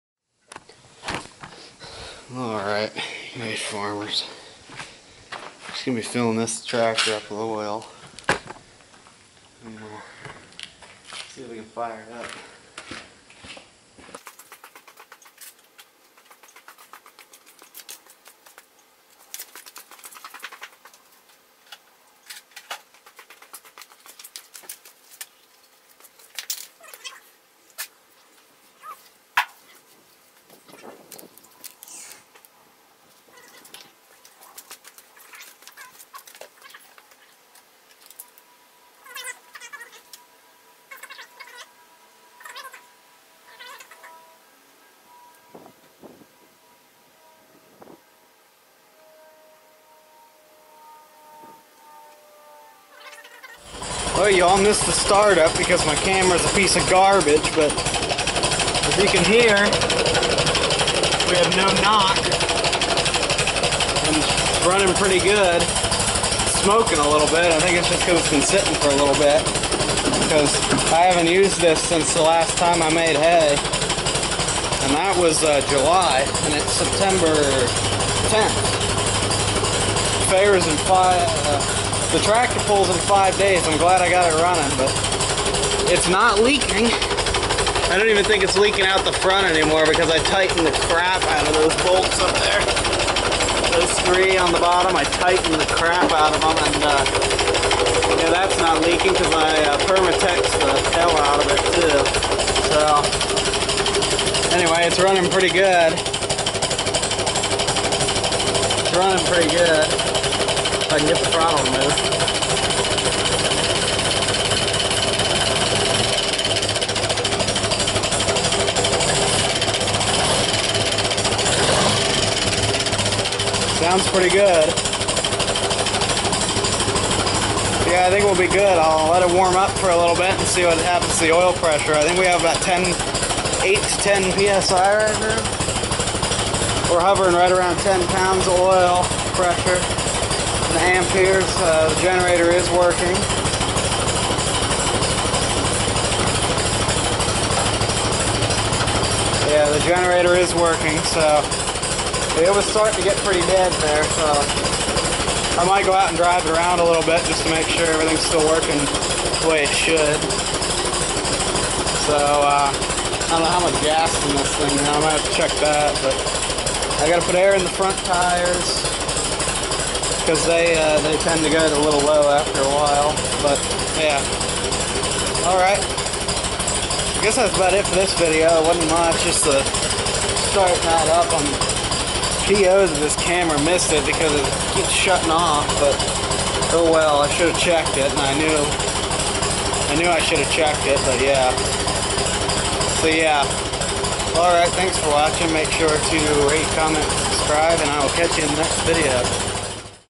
The 48 CO OP Lives
Got the tractor running again, still has a knock at idle, but Im going to do some other major work on it this winter, so it will be solved then.